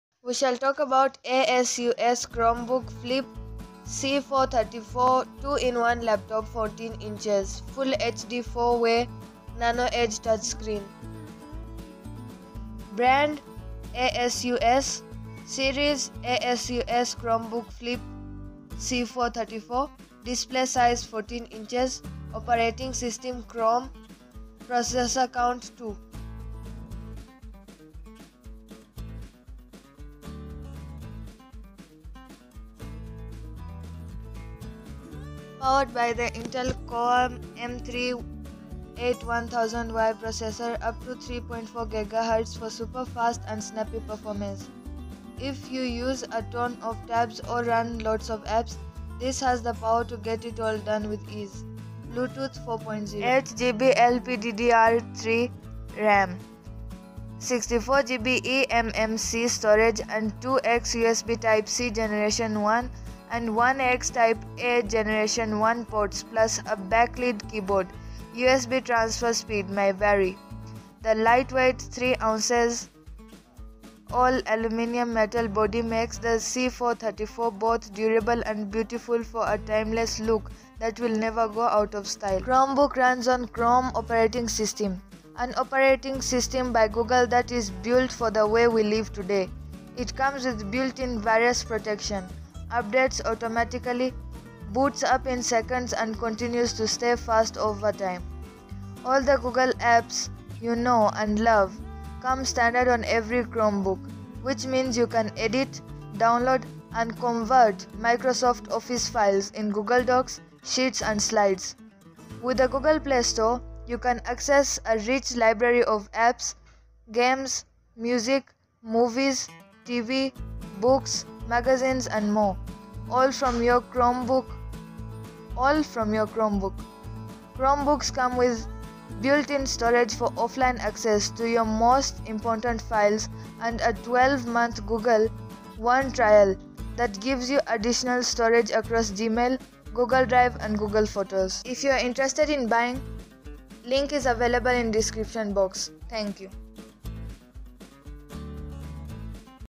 ASUS Chromebook 2-In-1 Laptop | Cool Innovative Marketing.
Hello viewers, today we shall talk about ASUS Chromebook 2-In-1 Laptop.

Details about ASUS Chromebook 2-In-1 Laptop.
Brand ASUS
Series Asus Chromebook Flip C434
Display Size 14 Inches
Operating System Chrome OS

14" Touchscreen Full HD 1920x1080 4-way Nano Edge display featuring ultra-narrow bezels (5mm thin) around each side of the display that allows for a 14" screen to fit in the body of a 13" laptop footprint.
The Full HD display has a durable 360-degree hinge that can be used to flip the touchscreen display to tent, stand, and tablet mode.
Powered by the Intel Core m3-8100Y Processor (up to 3. 4 GHz) for super-fast and snappy performance. If you use a ton of tabs or run lots of apps, this has the power to get it all done with ease. Bluetooth 4.0.

8GB LPDDR3 RAM; 64GB eMMC storage and 2x USB Type-C (Gen 1) and 1x Type-A (Gen 1) ports plus a backlit keyboard (USB Transfer speed may vary.
The lightweight (3 lbs.) all-aluminum metal body makes the C434 both durable and beautiful for a timeless look that will never go out of style.
Chromebook runs on Chrome OS - an operating system by Google that is built for the way we live today. It comes with built-in virus protection, updates automatically, boots up in seconds and continues to stay fast over time.
All the Google apps you know and love come standard on every Chromebook, which means you can edit, download, and convert Microsoft Office files in Google Docs, Sheets and Slides.
With the Google Play Store, you can access a rich library of apps, games, music, movies, TV, books, magazines, and more, all from your Chromebook.
Chromebooks come with built-in storage for offline access to your most important files and a 12-month Google One trial that gives you additional storage across Gmail, Google Drive and Google Photos.